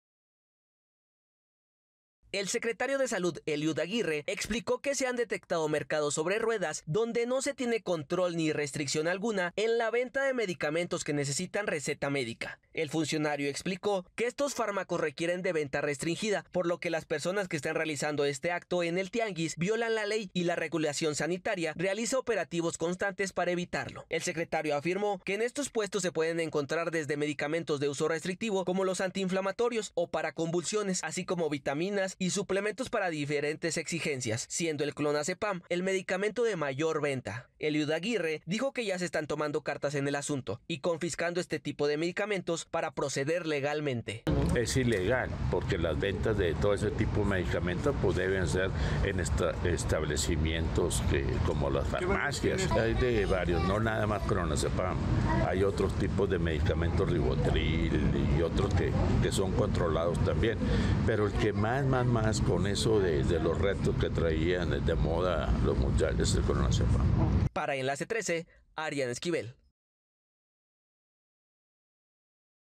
Clonazepam: el medicamento que más se vende de manera ilegal
Coahuila | 💊💰 Detectan venta ilegal de medicamentos en mercados sobre ruedas, donde el clonazepam es uno de los más frecuentes, por lo que la Secretaría de Salud continuará realizando operativos para el control de venta.